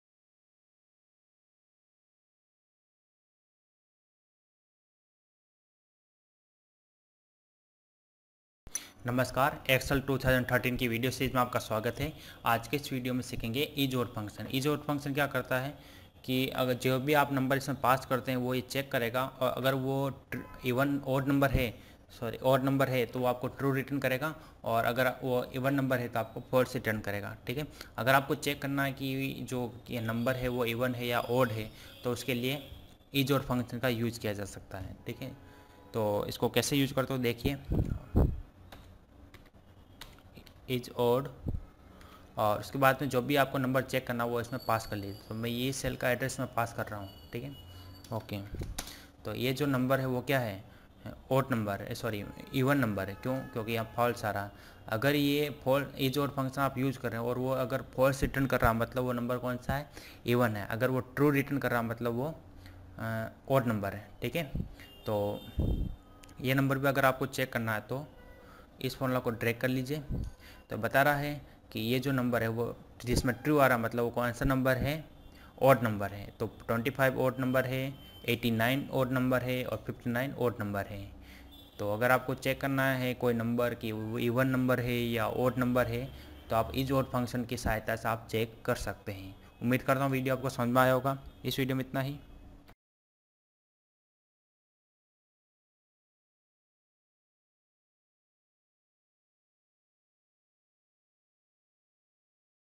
EXCEL 2013 Tutorial 78 in HINDI - ISODD
In this video, i will show you, how to use 'ISODD()' function in excel 2013. ISODD() returns true if the number is ODD.

Syntax :-

=ISODD(number)